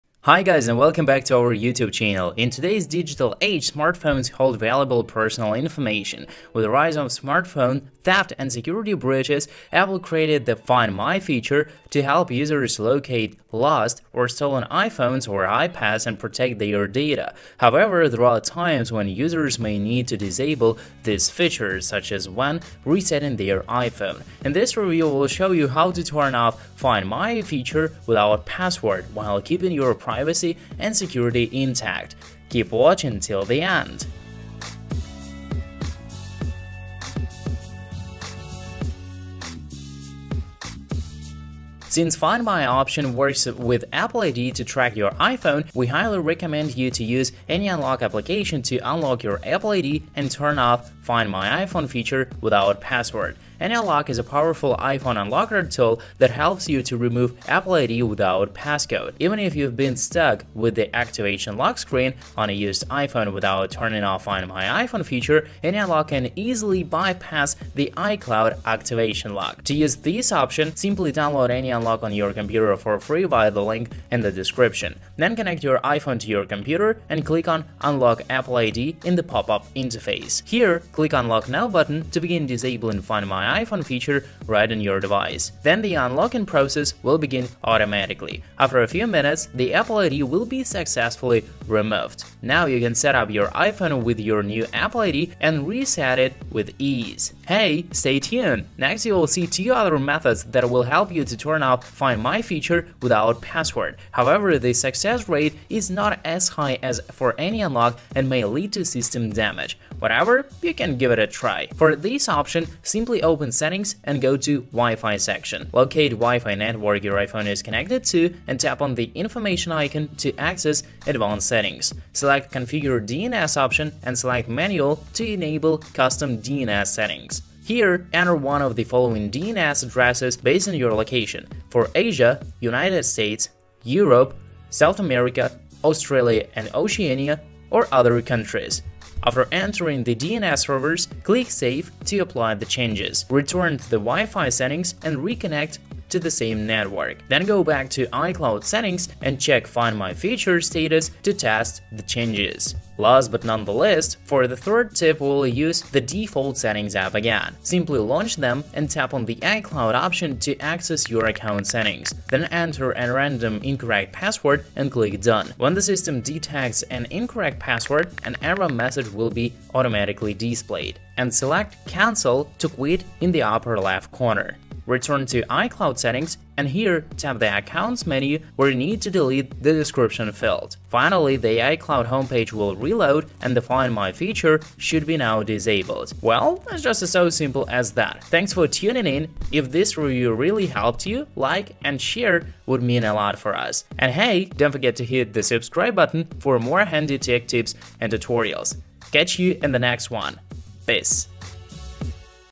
[NEW] How to Turn Off Find My iPhone without Password | Remove Apple ID | 3 Ways
In today's digital age, smartphones hold valuable personal information. With the rise of smartphone theft and security breaches, Apple created the "Find My iPhone" feature to help users locate lost or stolen iPhones and protect their data. However, there are times when users may need to disable this feature, such as when resetting their iPhone. In this video, we'll show you how to turn off "Find My iPhone" without password, while keeping your privacy and security intact.
0:00 Intro
0:40 Method 1. Turn off Find My iPhone without Password via Apple ID Removal
1:54 Method 2. Turn off Find My iPhone via DNS
2:34 Method 3. Turn off Find My iPhone by deleting your account